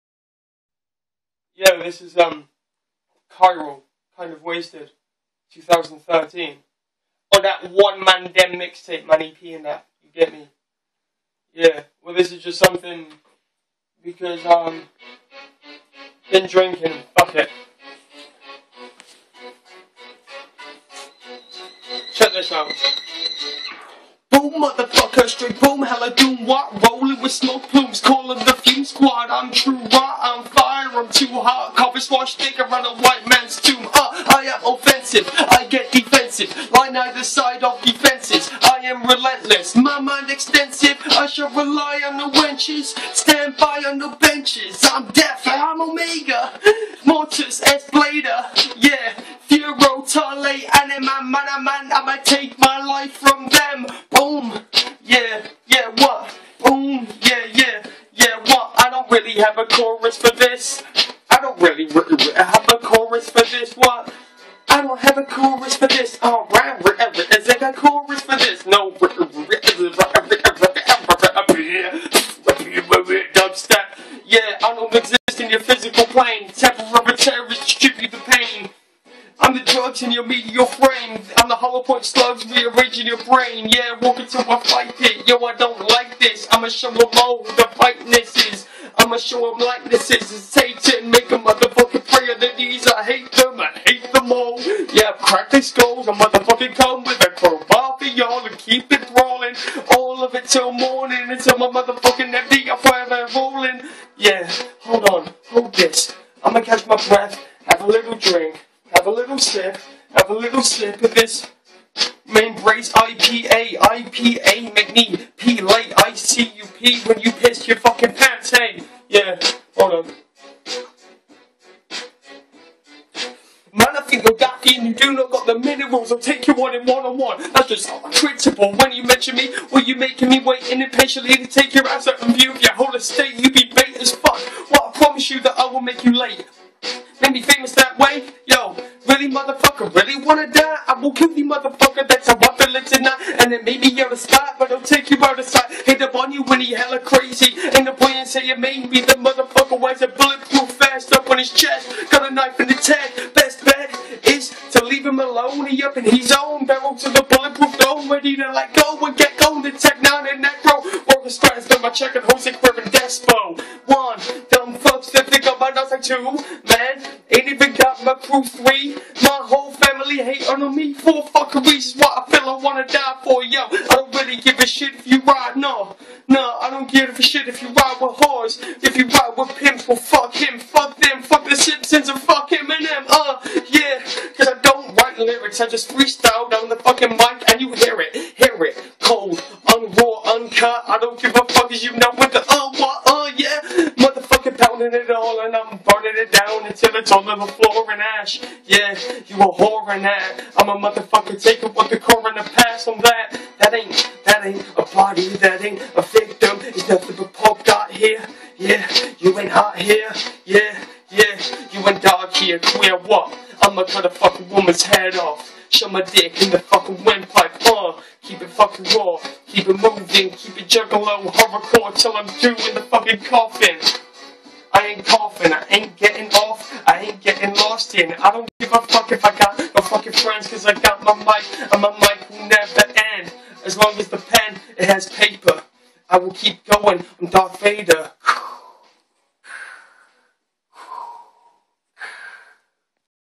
Not very sober VIDEO FREESTYLE - Khyral - Juggalo rap
Just a random freestyle after a beer and shit